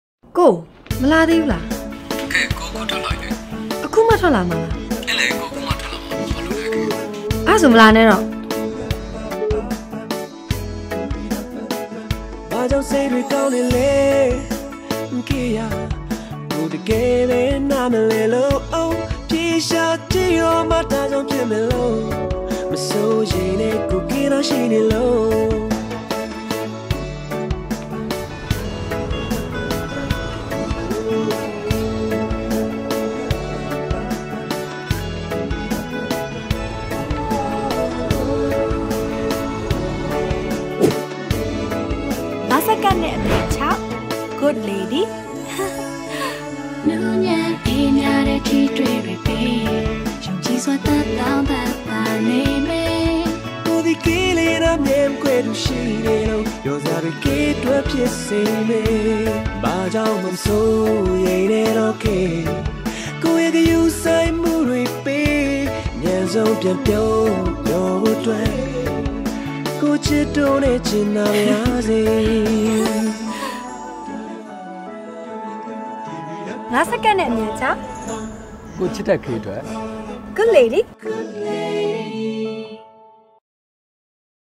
Good Lady TV Commercial
Director -  Academy Win Lwin Htet
Starring - Paing Takhon + Khin Wint Wah
Presented by HD Production Team
Credit